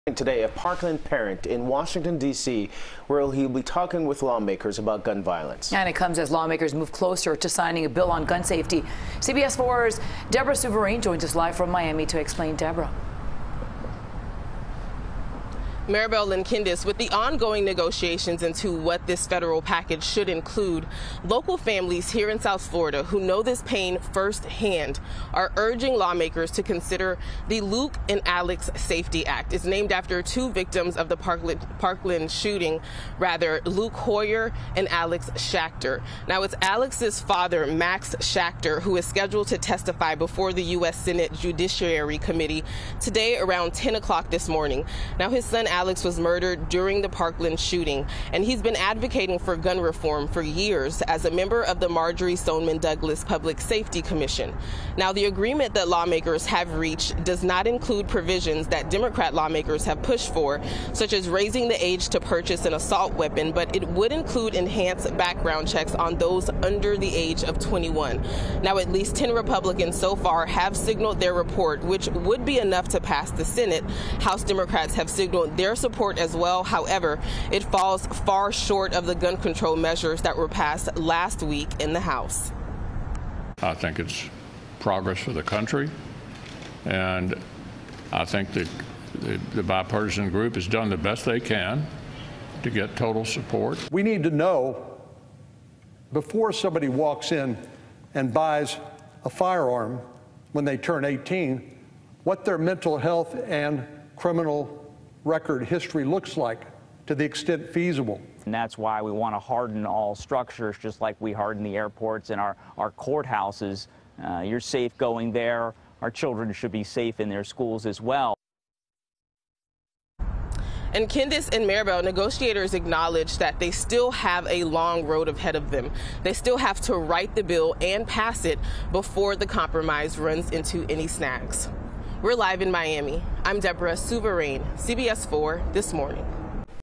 Parkland parent to address Congress on gun violence
Parkland parent Max Schachter will be addressing lawmakers in Washington, D.C., about gun reform.